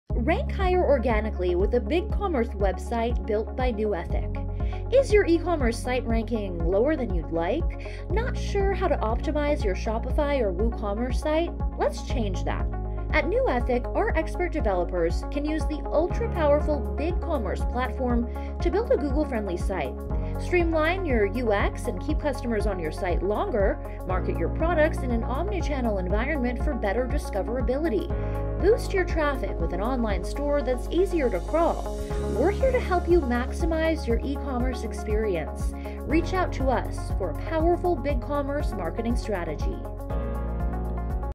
Rank Higher Organically With A BigCommerce Website Built By Nuethic
Is your e-commerce site ranking lower than you’d like? Not sure how to optimize your Shopify or WooCommerce site?

Let’s change that. At NuEthic, our expert developers can use the ultra-powerful BigCommerce platform to build a Google-friendly site.
Streamline your UX and keep customers on your site longer.
Market your products in an omnichannel environment for better discoverability.

Boost your traffic with an online store that’s easier to crawl.
We’re here to help you maximize your e-commerce experience.